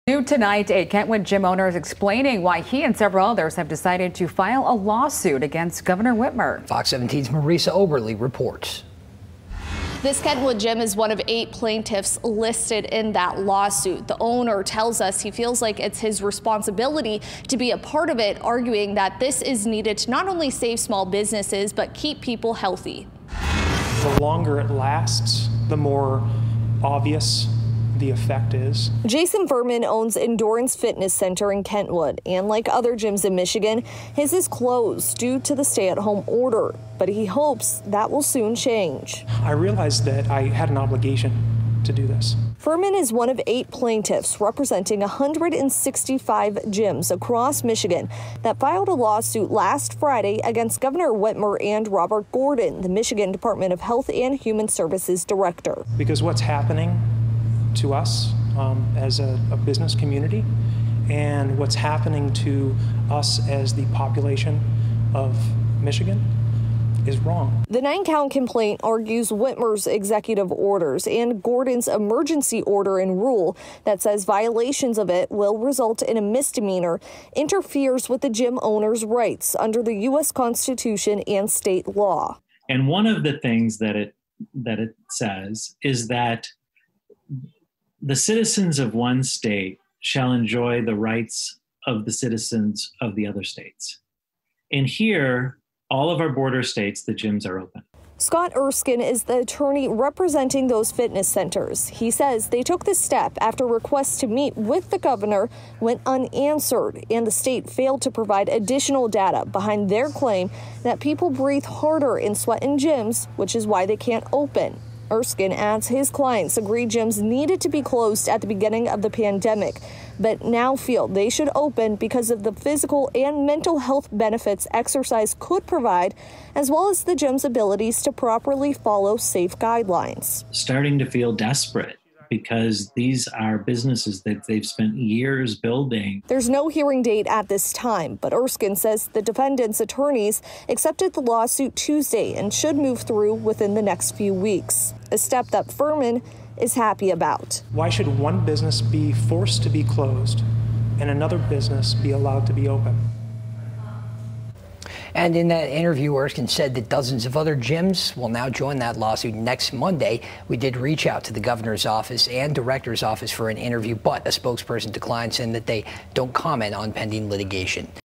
gyms suing governor whitmer
Michigan gyms are suing Gov. Whitmer and Director Gordon over the stay-at-home order